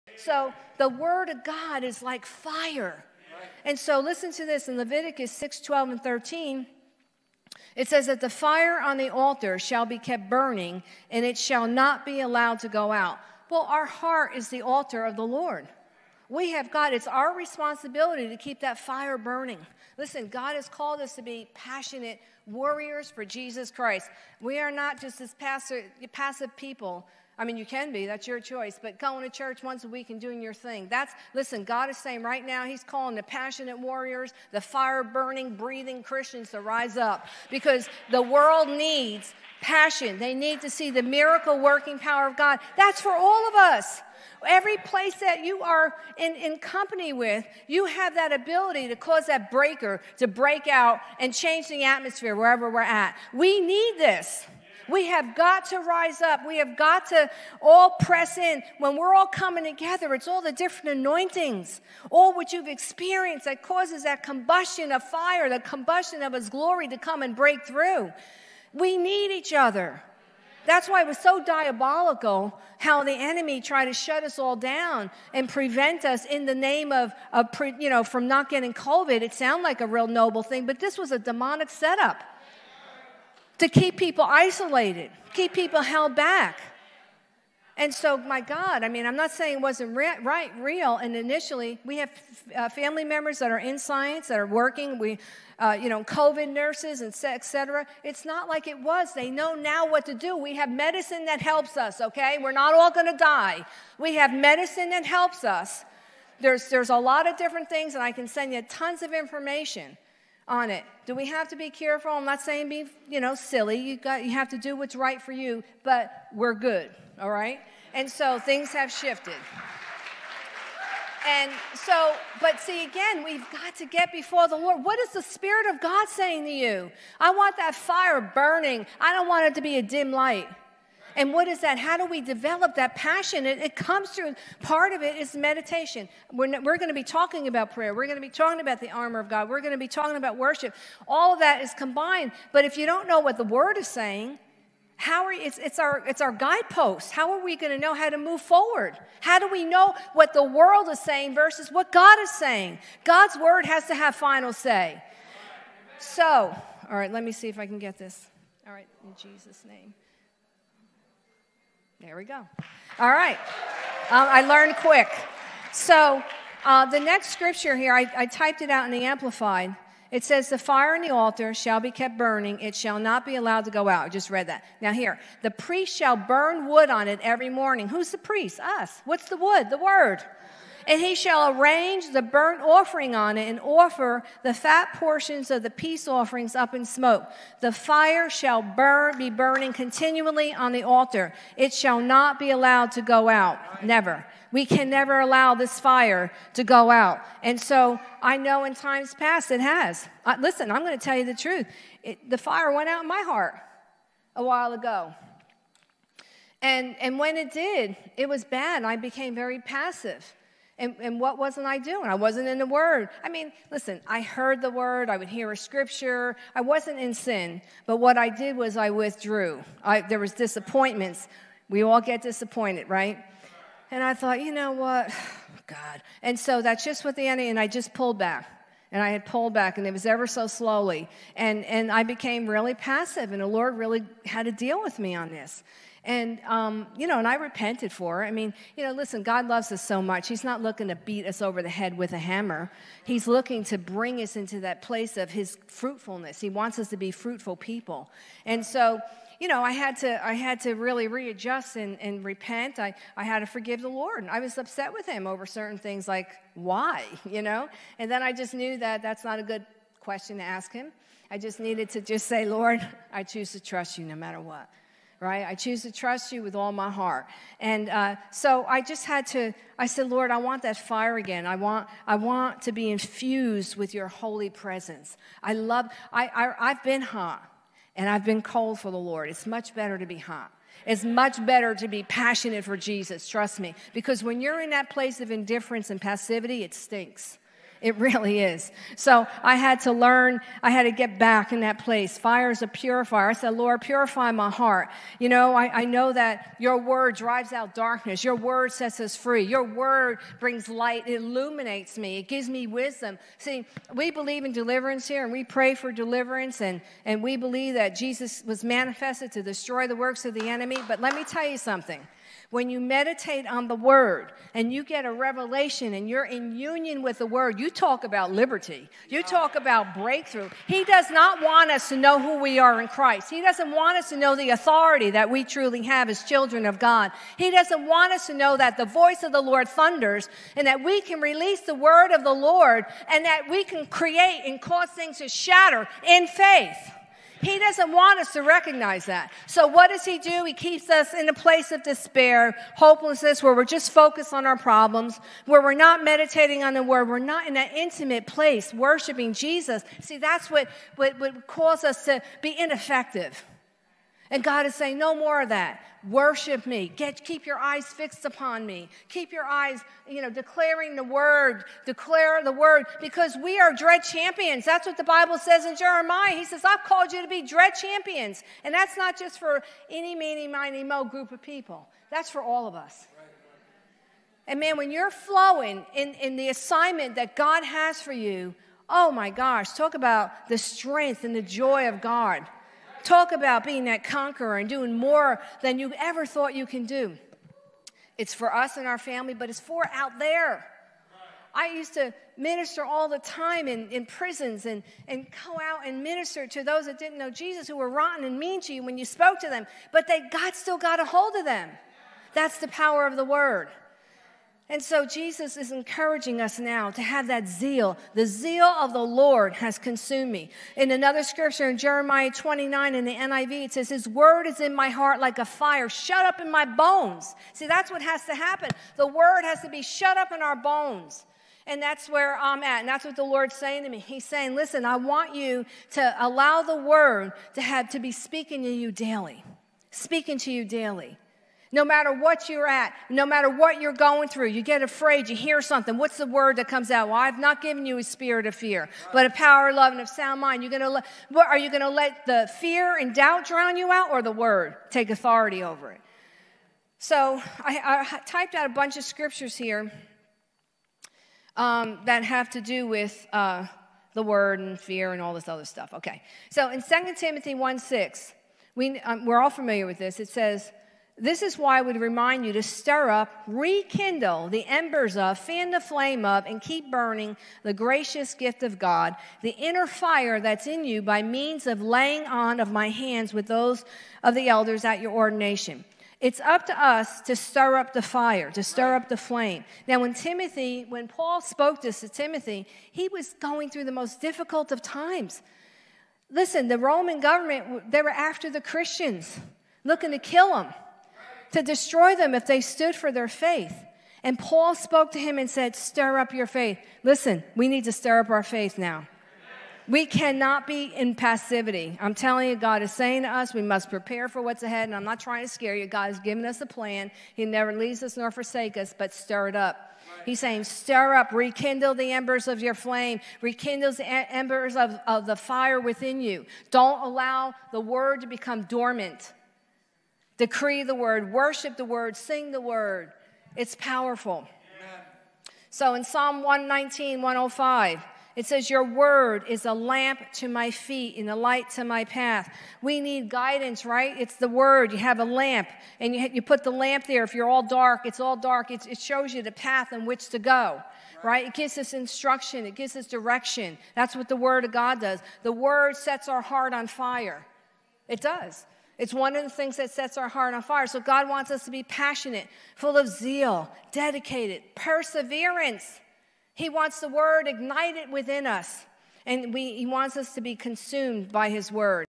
Trisha Roselle: Fanning the Flame of Burning Hearts (2 Timothy 1:6)
Quotes: “The word of God is like fire. Leviticus 6:12-13, it says that "The fire on the altar shall be kept burning; and it shall not be allowed to go out." Our heart is the altar of the Lord. We have God; it's our responsibility to keep that fire burning. God has called us to be passionate warriors for Jesus Christ. We are not meant to be a passive group of people. He's calling forth the passionate warriors, the fire-burning, fire-breathing Christians to rise up. The world needs to see passion, they need to see the miracle working power of God. That's for all of us.”

Be blessed.

Every place that you are in company with, you have that ability to cause that breaker to break out and change the atmosphere. We need this. We have got to rise up, we have got to all press in. When we're all coming together, it's all the different anointings, all which you've experienced that causes that combustion of fire, the combustion of His glory to come and break through.

We need each other. That's why it was so diabolical how the enemy tried to shut us all down and prevent us from gathering during COVID. It sounded like a real noble thing, but this was a demonic setup to keep people isolated, keep people held back.

What is the Spirit of God saying to you? I want that fire burning, I don't want it to be a dim light.
How do we develop that passion? It comes through, part of it is meditation. We're going to be talking about prayer, we're going to be talking about the armor of God, we're going to be talking about worship, all of that is combined. But, if you don't know what the word is saying, how are we going to know how to move forward? How do we know what the world is saying, versus what God is saying? God's word has to have final say.

The Lord really had to deal with me about pulling back out of disappointment. I repented for it. God loves us so much! He's not looking to beat us over the head with a hammer, He's looking to bring us into that place of His fruitfulness. He wants us to be fruitful people. I had to really readjust and repent. I had to forgive the Lord because I was upset with Him over certain things like. I just needed to just say, “Lord, I choose to trust you no matter what. I choose to trust you with all my heart.” I said, “Lord, I want that fire again. I want to be infused with your Holy presence.”

I've been hot, and I've been cold for the Lord. It's much better to be hot. It's much better to be passionate for Jesus, trust me. Because when you're in that place of indifference and passivity, it stinks. I had to get back in that place where the fire could purify me. I said Lord, purify my heart, I know that your word drives out darkness. Your word sets us free. Your word brings light, it illuminates me. It gives me wisdom.  See, we believe in deliverance here, and we pray for deliverance, and we believe that Jesus was manifested to destroy the works of the enemy (1 John 3:8). When you meditate on the word, and you get a revelation, and you're in union with the word, you talk about liberty. You talk about breakthrough.

The enemy does not want us to know who we are in Christ. He doesn't want us to know the authority that we truly have as children of God. He doesn't want us to know that the voice of the Lord thunders, and that we can release the word of the Lord, and that we can create and cause things to shatter, in faith. He doesn't want us to recognize that. So what does he do? He keeps us in a place of despair, hopelessness, where we're just focused on our problems. Where we're not meditating on the word, we're not in that intimate place, worshiping Jesus.

See, that's what calls us to be ineffective. And God is saying, no more of that. Worship me, keep your eyes fixed upon me. Declare the word. Because we are dread champions. When you're flowing in the assignment that God has for you, you receive the strength and the joy of God. Talk about being that conqueror and doing more than you've ever thought you can do. It's for us and our family, but it's also for the culture. I used to minister all the time in prisons, and go out and minister to those that didn't know Jesus, who were rotten and mean to you when you spoke to them. But God still got a hold of them. That's the power of the word.